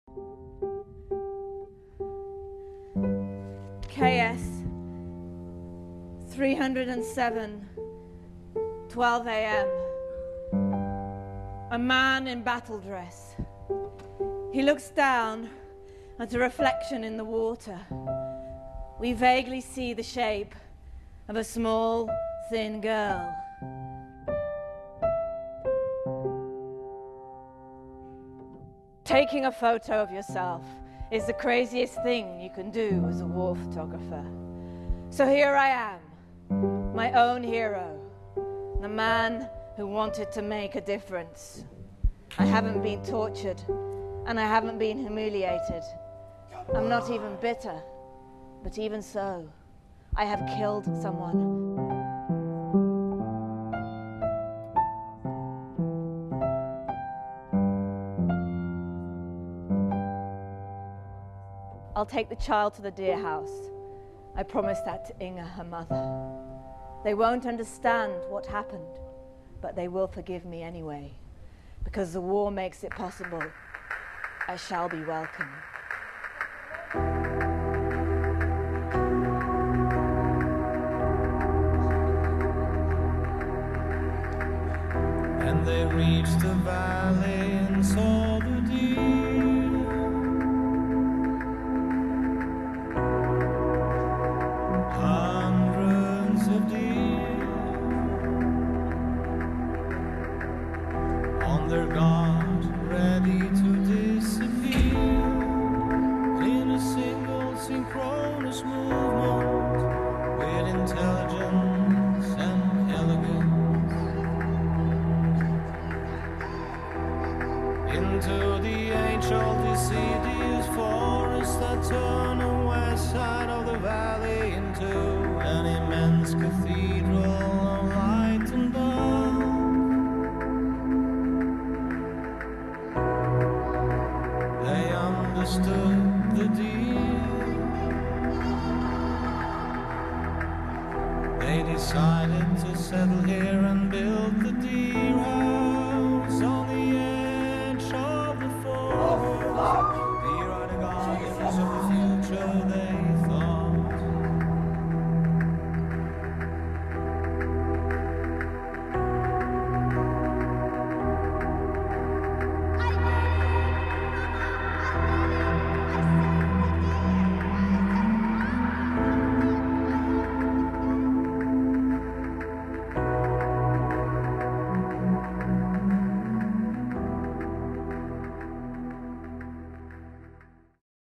Jan Lauwers & Needcompany | The Deer House
In The Deer House Jan Lauwers mixes two storylines, thus creating a surreal and absurd fable about the present. The Deer House is the concluding part of the Sad Face / Happy Face trilogy, comprising three stories about human nature.